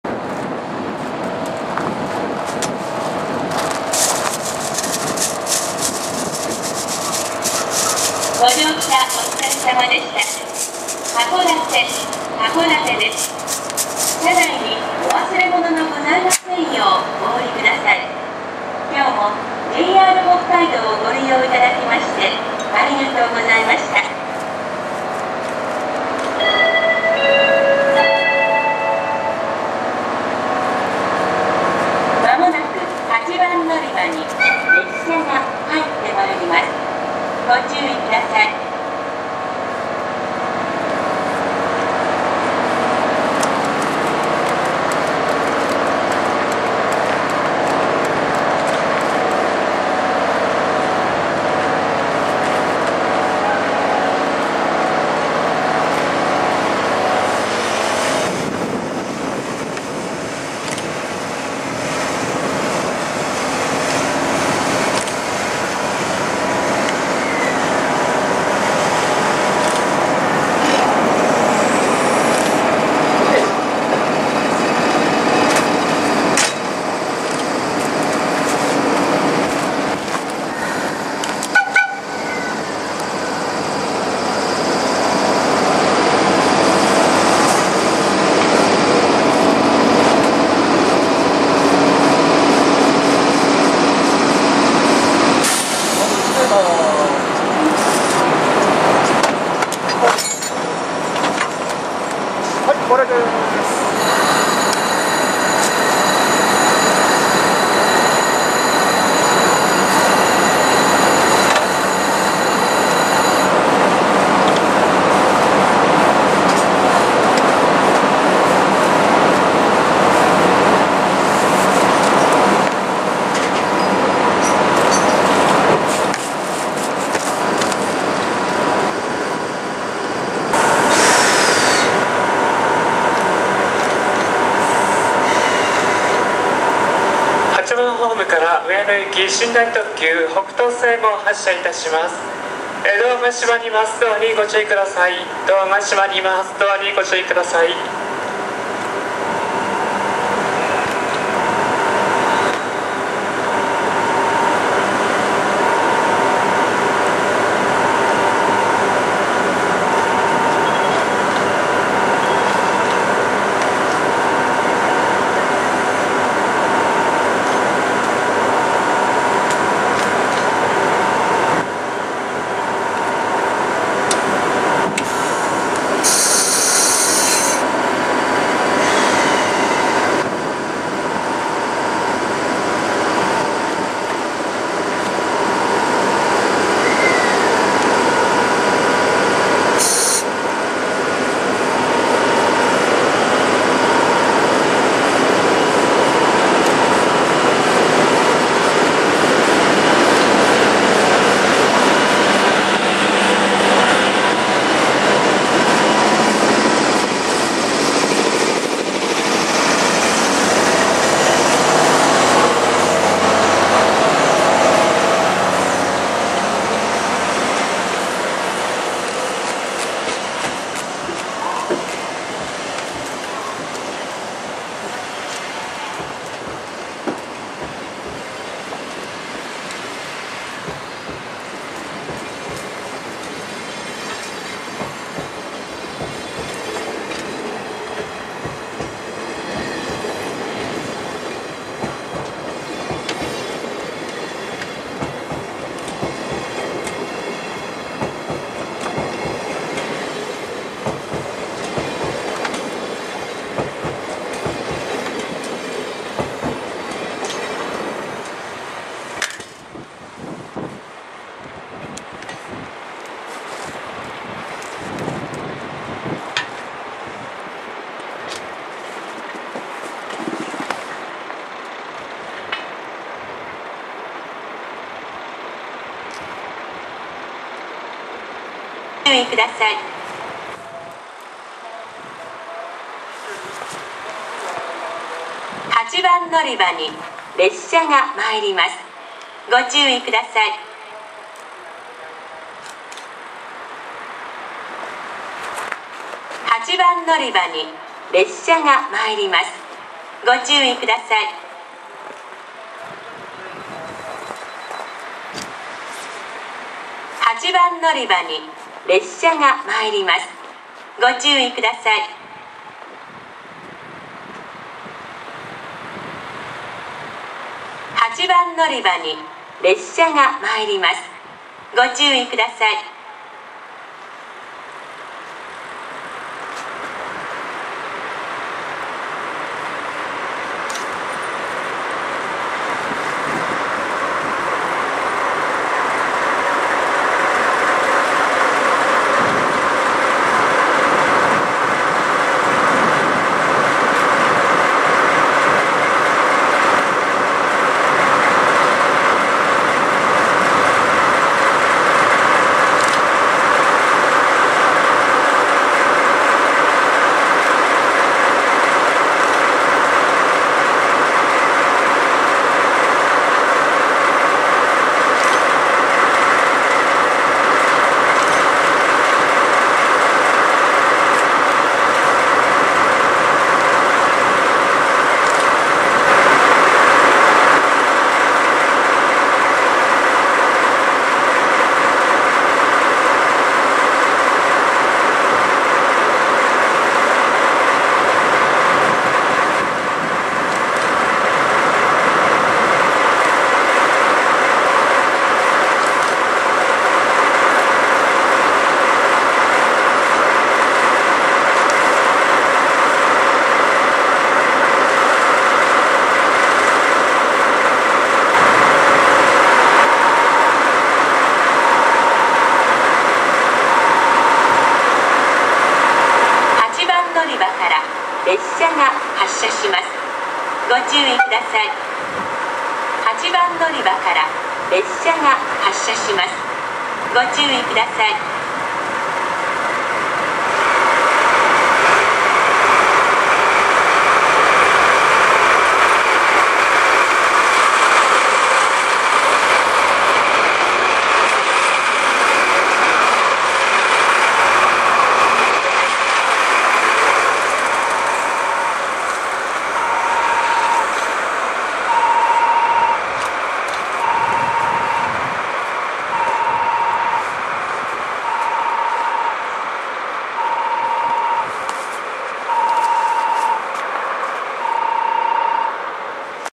【連結】北斗星 機関車付け替え→発車・ＤＤ５１回送発車
撮影日　　2013年1月9日　　　函館駅にて

札幌発、上野行きの寝台特急北斗星が函館駅到着後、当駅まで牽引してきたＤＤ５１を切り離し、青森方にＥＤ７９を連結します
北斗星が発車した後、この線で待機していたＤＤ５１が函館運輸所に回送されます
その際、入換信号機により、私の目の前で停車しました
しばらく撮影していると、運転士さんが前灯を消灯してくれるというサービス
また、警笛を軽く鳴らして発車しました
心優しい運転士さんに感謝です！